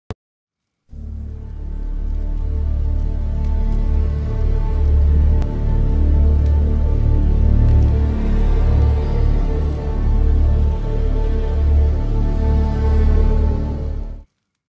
Heroic Intro By Me
This is the first time I have ever made a intro from scratch and I think its a good start.